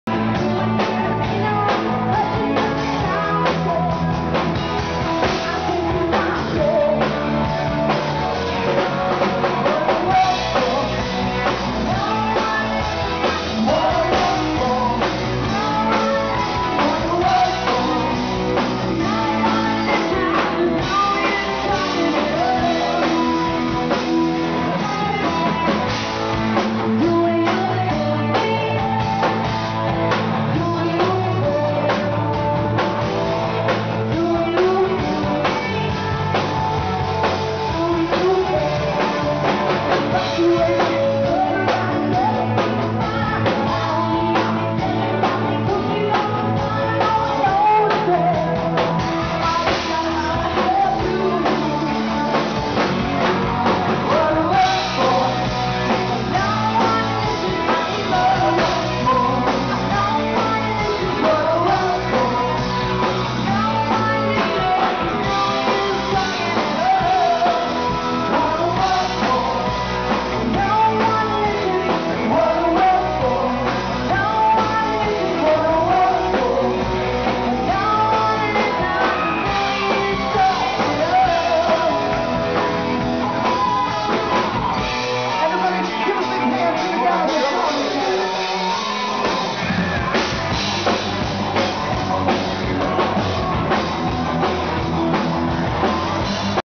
Live80 Rocks Cool River Dallas!
John Viola has been a friend for years. This band plays all 80s all the time with no breaks between songs. If you haven't yet, check out Live80!